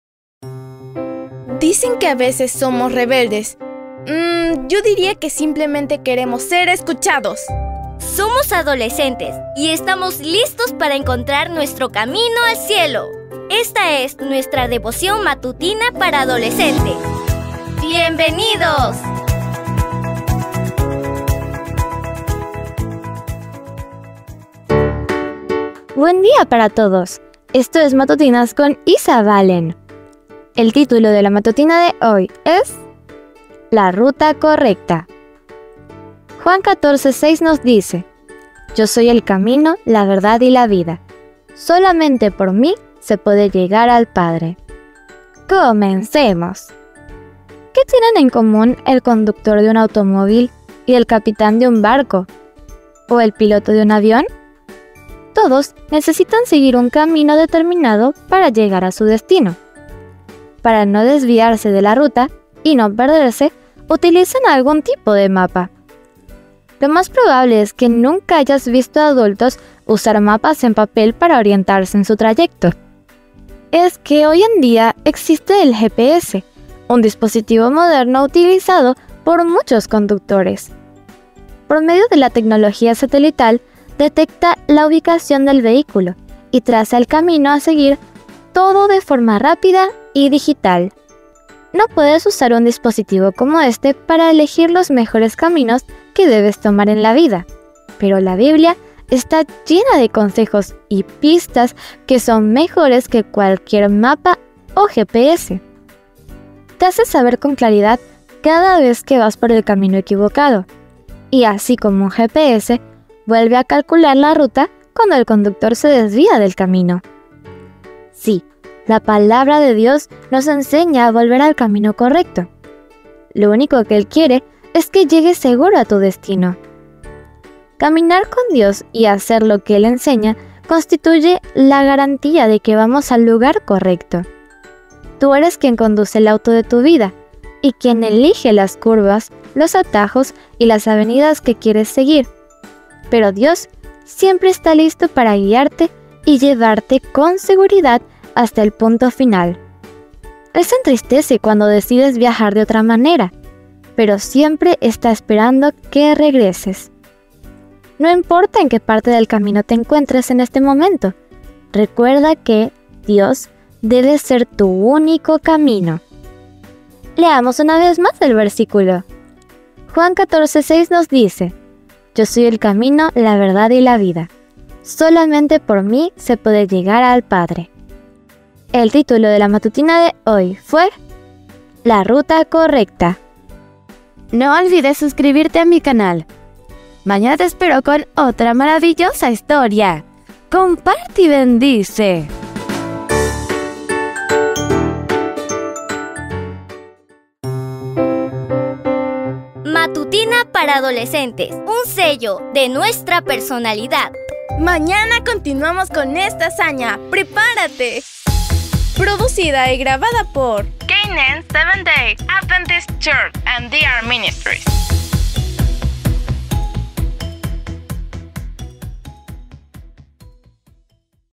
Matutina para Adolescentes - 29 de Junio del 2023
29 DE JUNIO
LA RUTA CORRECTA

"Yo soy el camino, la verdad y la vida. solamente por mí se puede llegar al Padre" (Juan 14:6).

¿Qué tienen en común el conductor de un automóvil, el capitán de un barco y el piloto de un avión? Todos necesitan seguir un camino determinado para llegar a su destino. Para no desviarse de la ruta y no perderse, utilizan algún tipo de mapa. Lo más probable es que nunca hayas visto a adultos usar mapas en papel para orientarse en su trayecto. Es que, hoy en día, existe el GPS, un dispositivo moderno utilizado por muchos conductores. Por medio de la tecnología satelital, detecta la ubicación del vehículo y traza el camino a seguir, todo de forma rápida y digital.
No puedes usar un dispositivo como este para elegir los mejores caminos que debes tomar en la vida, pero la Biblia está llena de consejos y pistas que son mejores que cualquier mapa o GPS. Te hace saber con claridad cada vez que vas por el camino equivocado y, así como un GPS, vuelve a calcular la ruta cuando el conductor se desvía del camino. Sí, la Palabra de Dios nos enseña a volver al camino correcto. Lo único que él quiere es que llegues seguro a tu destino.
Caminar con Dios y hacer lo que él enseña constituye la garantía de que vamos al lugar correcto. Tú eres quien conduce el automóvil de tu vida y quien elige las curvas, los atajos y las avenidas que quieres seguir, pero Dios siempre está listo para guiarte y llevarte con seguridad hasta el punto final. Él se entristece cuando decides viajar de otra manera, pero siempre está esperando que regreses. No importa en qué parte del camino te encuentras en este momento. Recuerda que
UNO CAMINA Y DIOS GUIA.